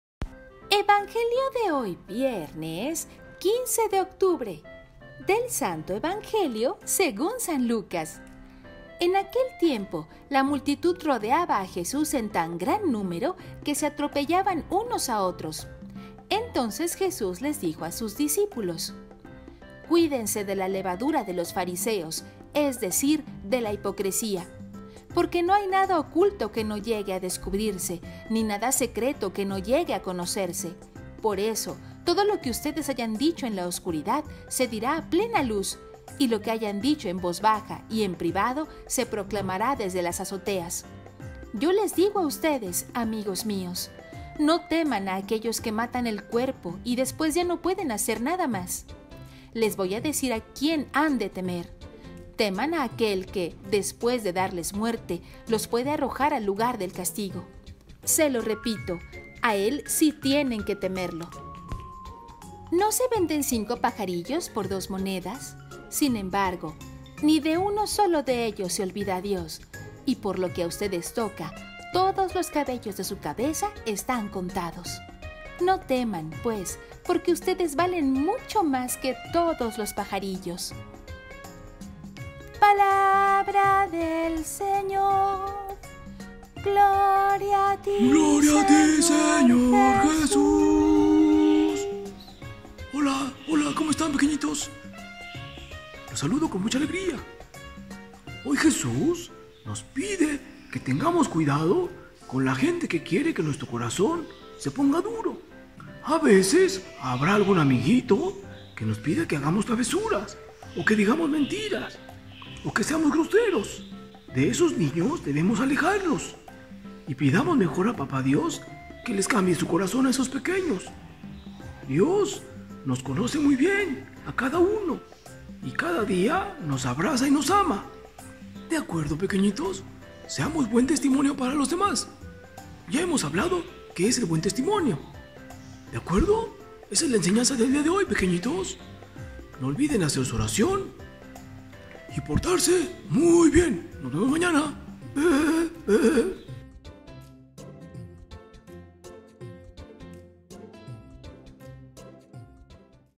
REFLEXIÓN DEL EVANGELIO PARA NIÑOS Viernes 15 10 21
Aprendamos a cuidar nuestro corazón evitando escuchar los malos consejos.